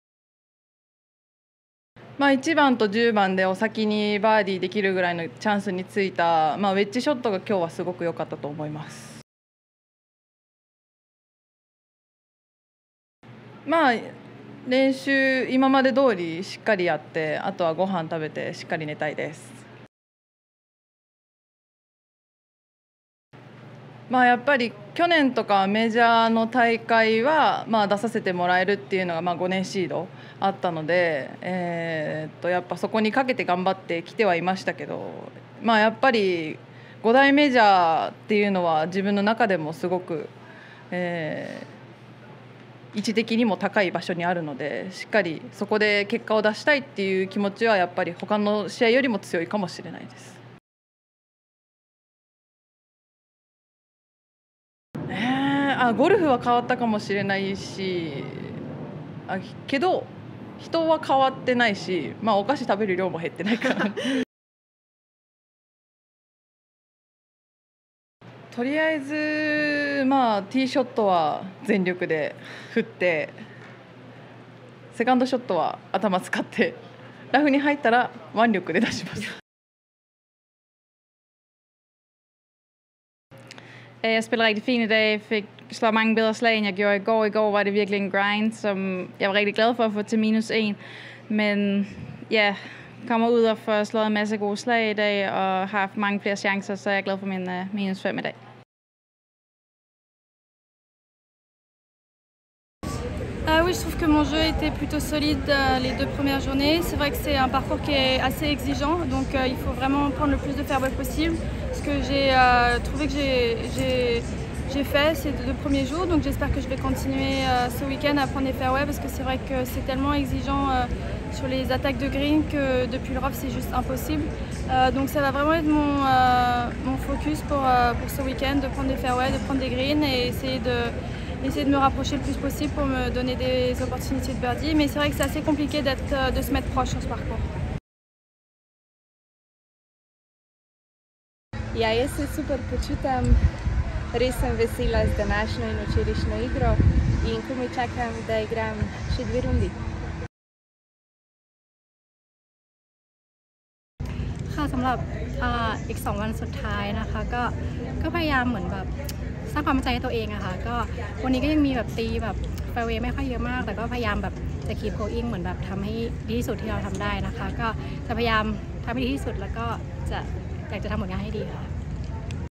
Reaction following the 2nd round of LPGA's Chevron Championship | Golf | Hinako Shibuno渋野日向子 ゴルフ 高尔夫
Reaction following the second round of the LPGA's Chevron Championship on Friday at Mission Hills.
Japan's Hinako Shibuno shot a 66 to take a one-stroke lead through 36 holes.